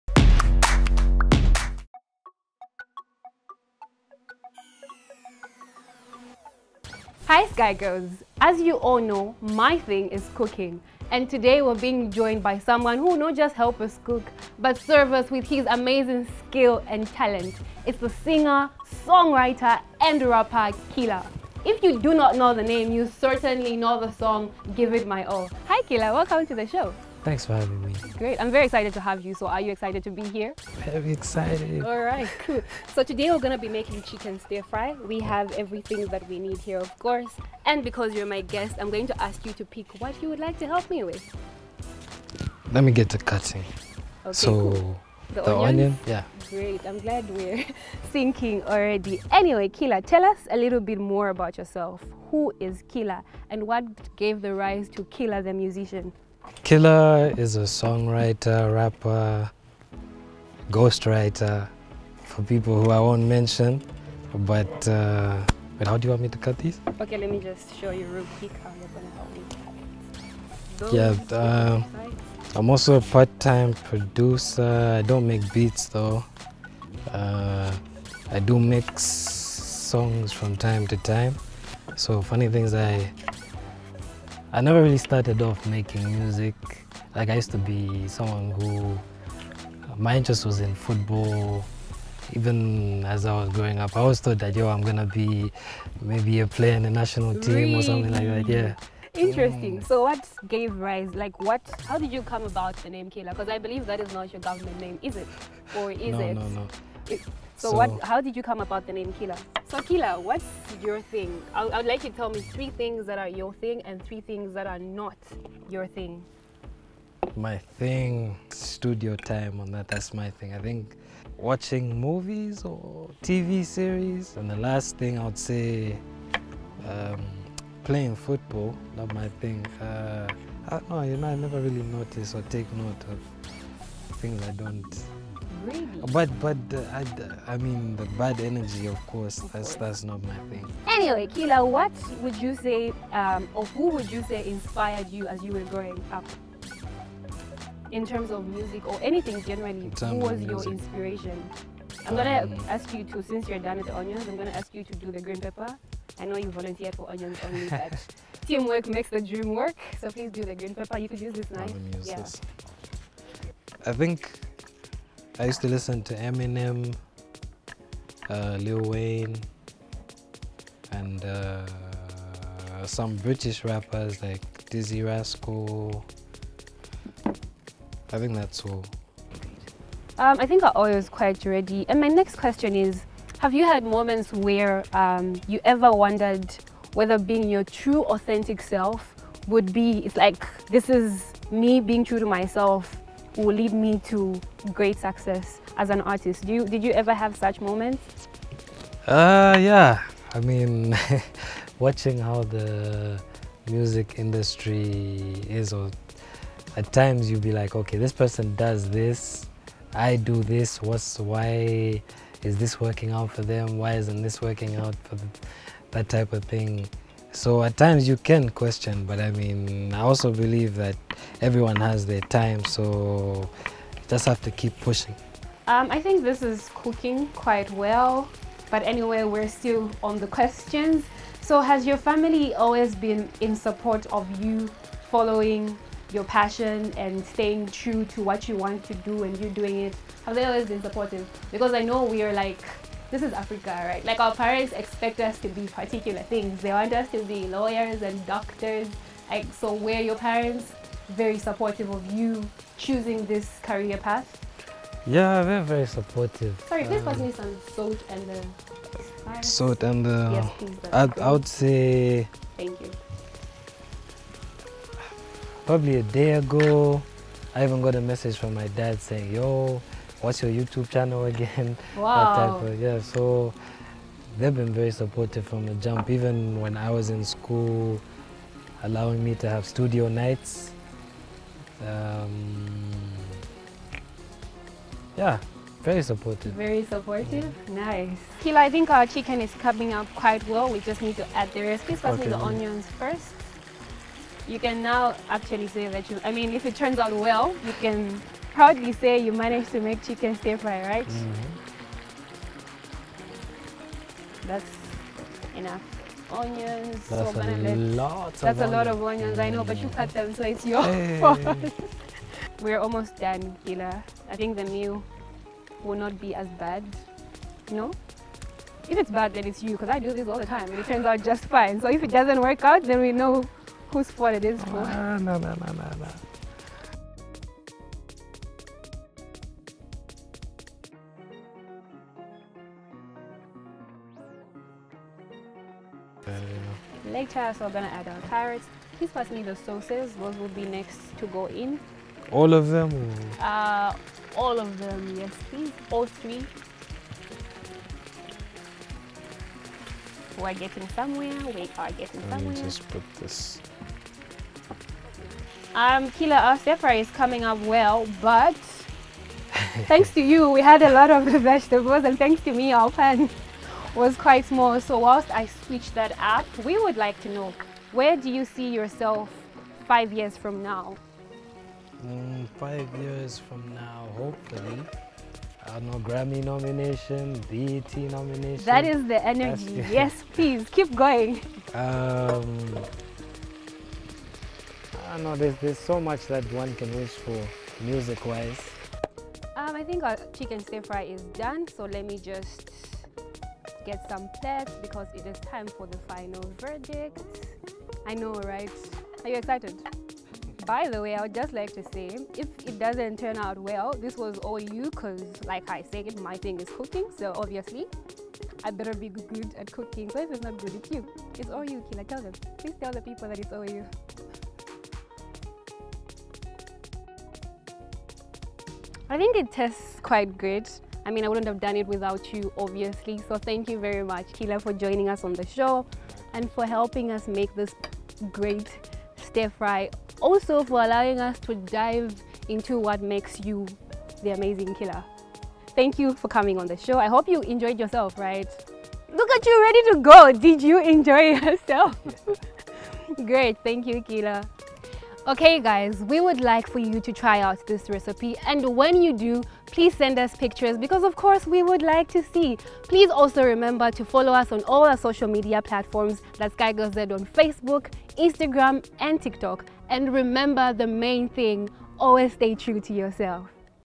Cooking With Audrey Ep 4 with Guest Killa
Audrey is back with special guest, singer and song writer Killa and they  are cooking up Chicken Stir Fry🥘